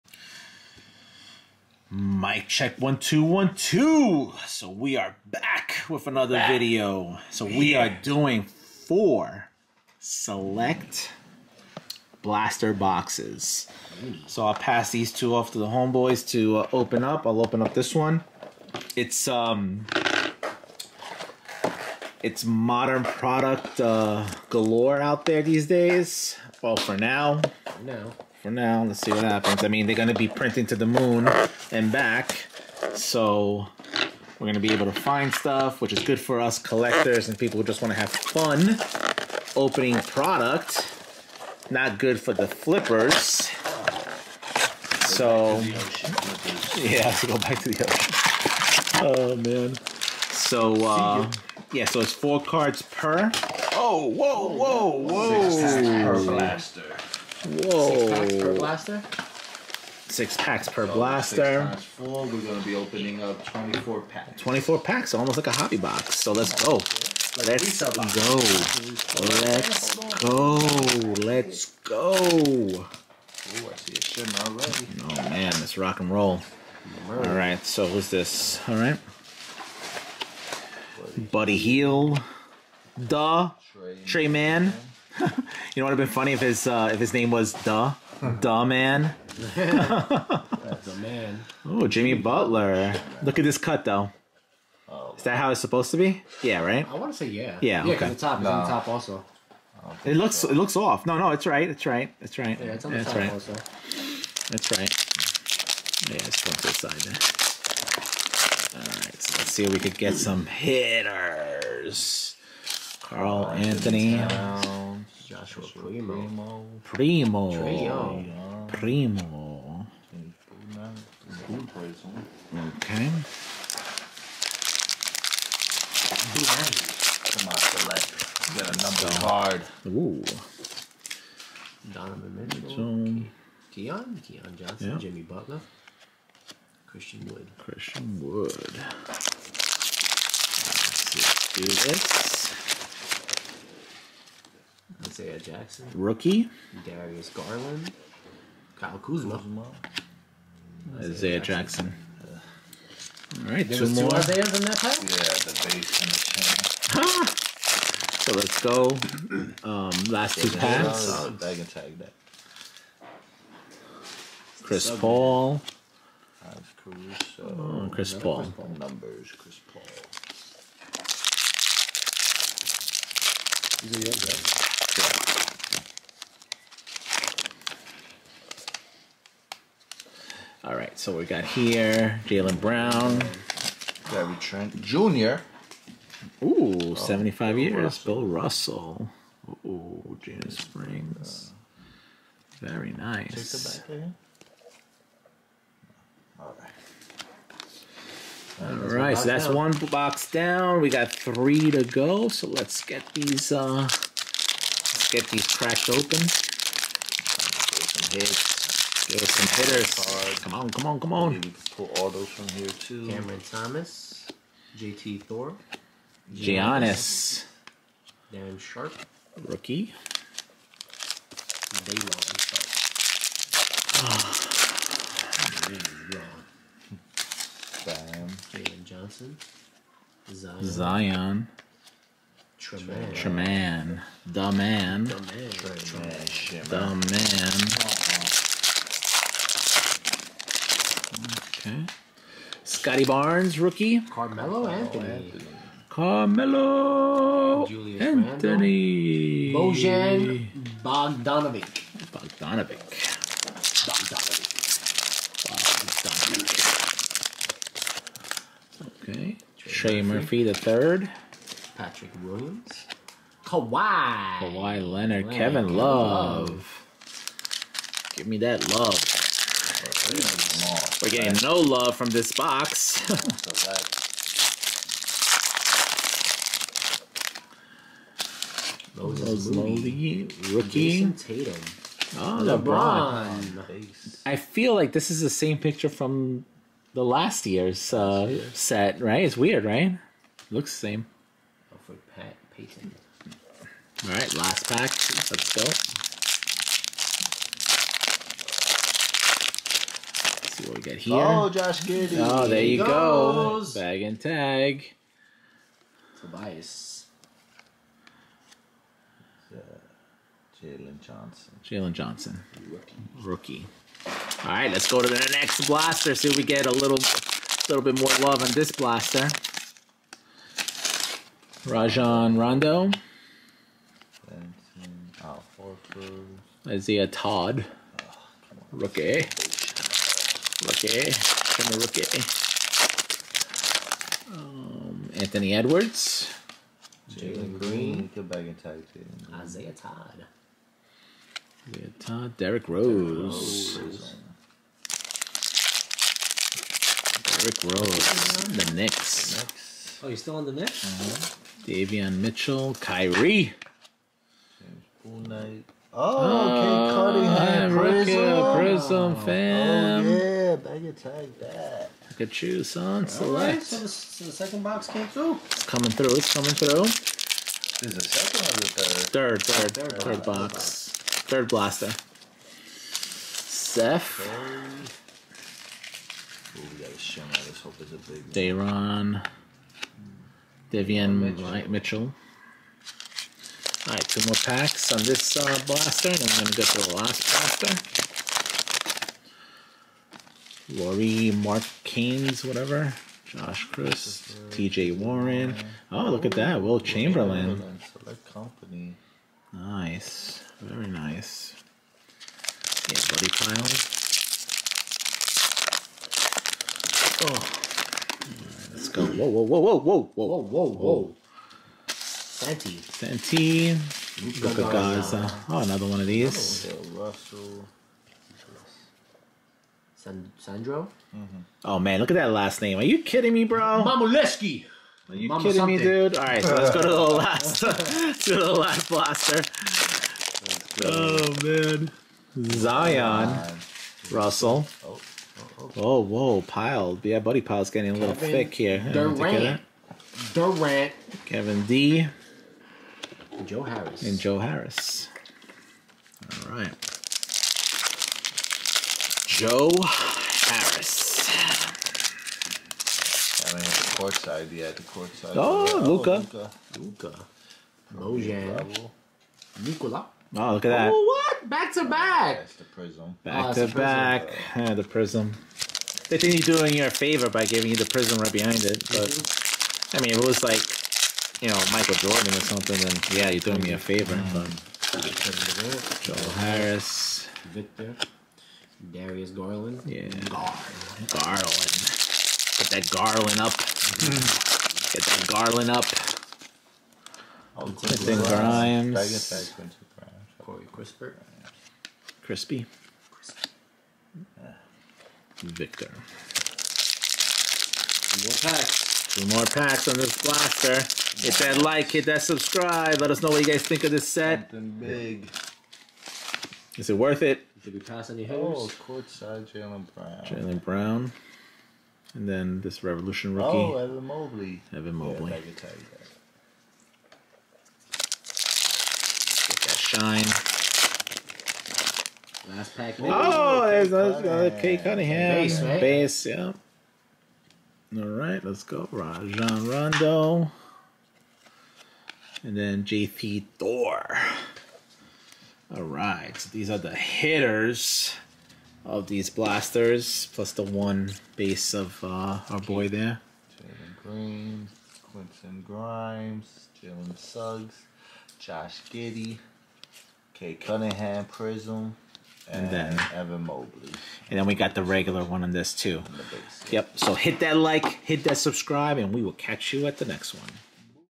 Select 21-22 Basketball Blaster Box Break Rare card hunt inserts numbered cards fun RIP
Junkwaxjunkies do a box break of 21 22 Select Basketball Blaster Box break. We are on the hunt for rare cards prizm parallels key players & future hall of famers.